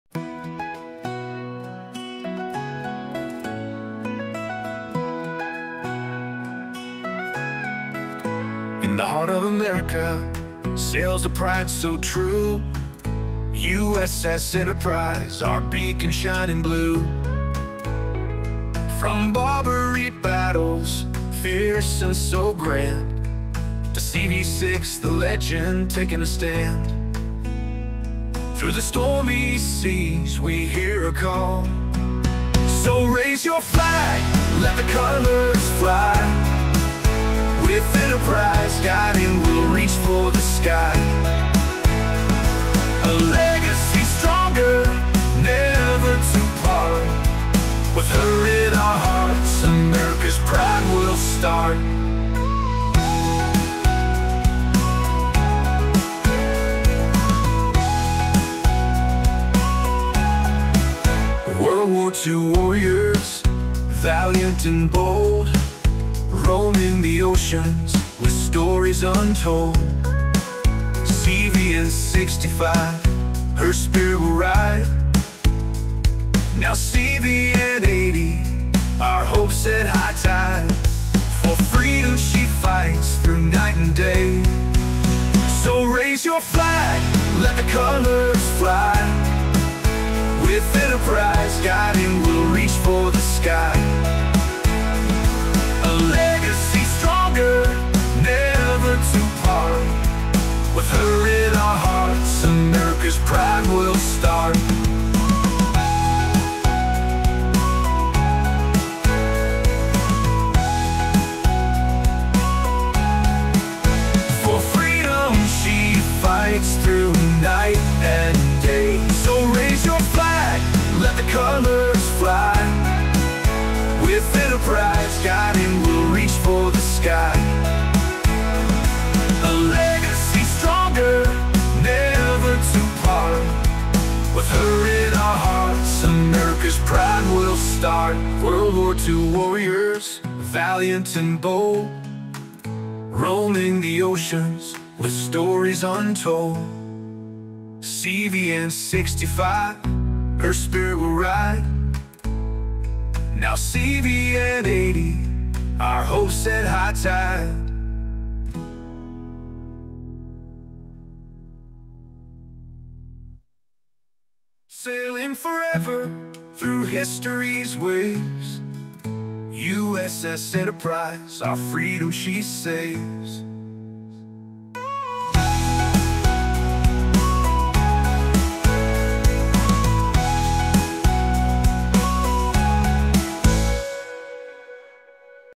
Asking Ai To Write A Hit Country Song For America Unofficial Flag Ship The Uss Enterprise
Music from
    ai generated by donna
ART FROM
  ai generated art work from ai art